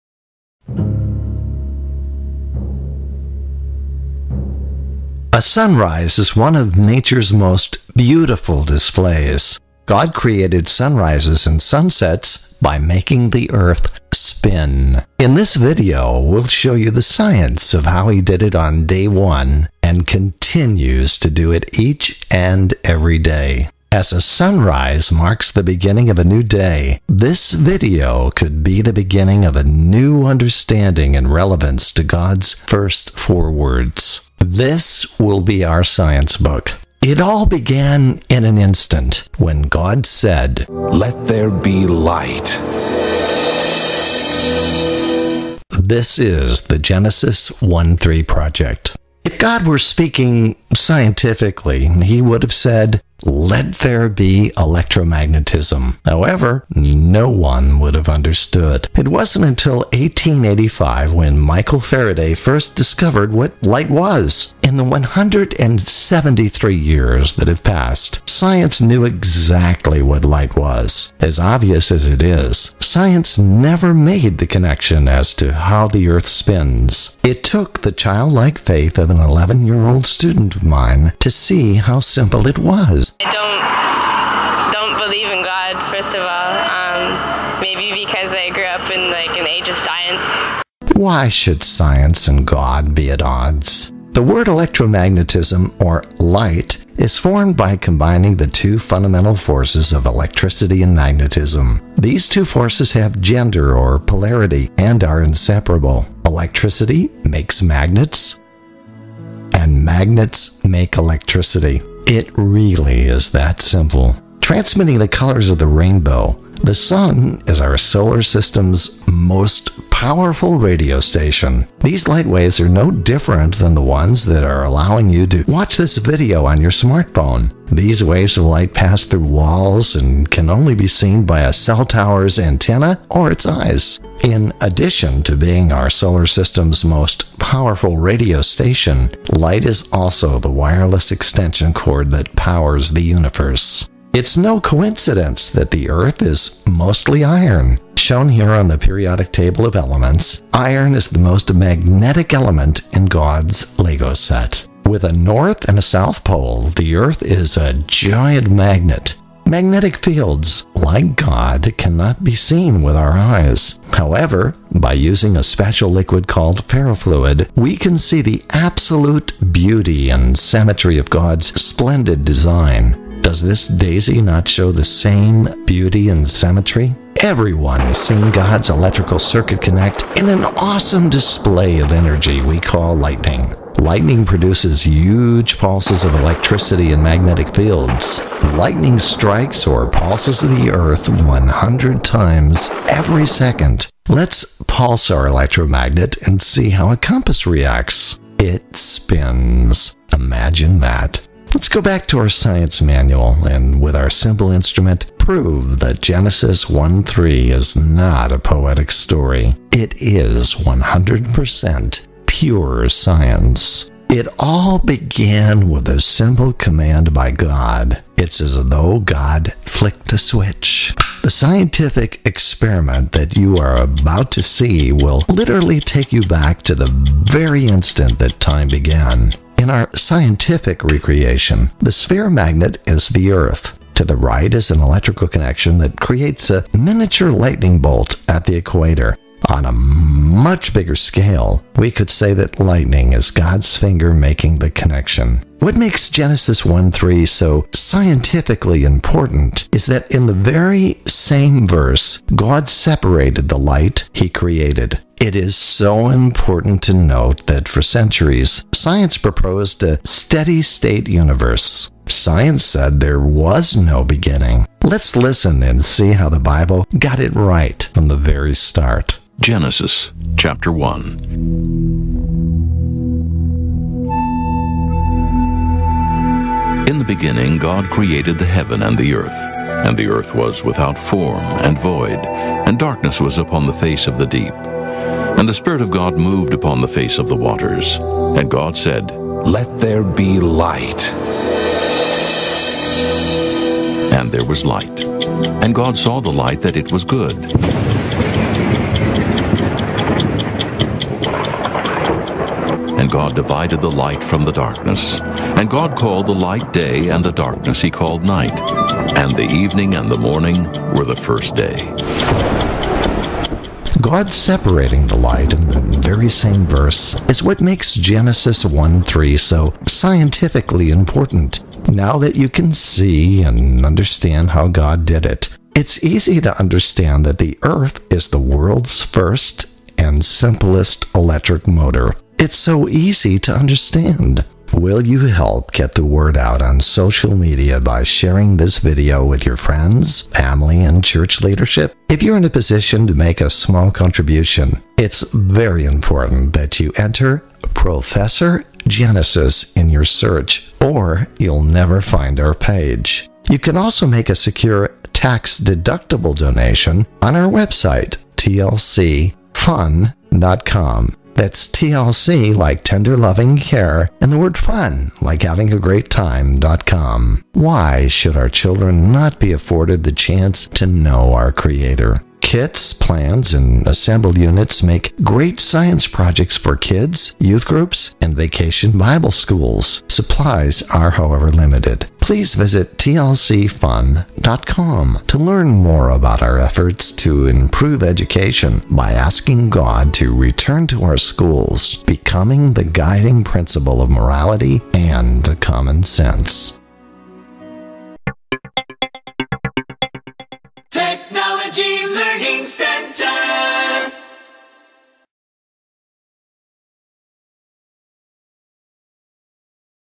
GENESIS 1:3 THE SCIENCE OF A SUNRISE
What makes Genesis 1:3 so scientifically important is in the very same verse where God created light He separated it into night and day.  We now know how He did it.  He made the Earth spin or rotate.  In the grand scheme of time, it has only been a matter of minutes since science discovered what light was. What you are about to see is not a theory.  In order to be science, it must be capable of being replicated.  Anyone with a bill of materials and some basic building skills can recreate the instant that time began.  Unlike the Hadron Collider which is 17 miles in length and cost well over 50 billion dollars, the Genesis device takes up only one cubic ft. of space and costs less than $200.  Here is how it works.